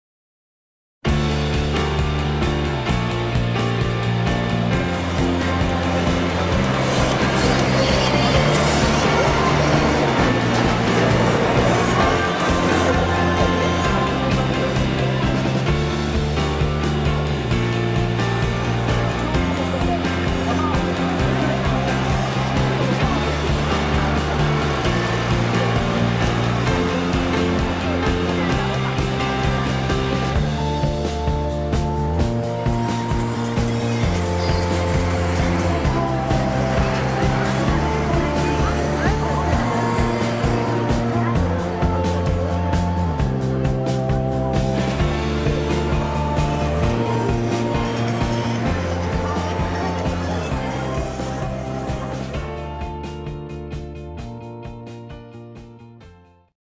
乃木坂46やBLACKPINKが出演する、9月2日開催 東京ガールズコレクション 2017 AUTUMN/WINTER の宣伝トラック
渋谷を走行する、2017年9月2日(土) に、乃木坂46やBLACKPINKが出演し、さいたまスーパーアリーナで開催される。「東京ガールズコレクション 2017 AUTUMN/WINTER」の宣伝トラック。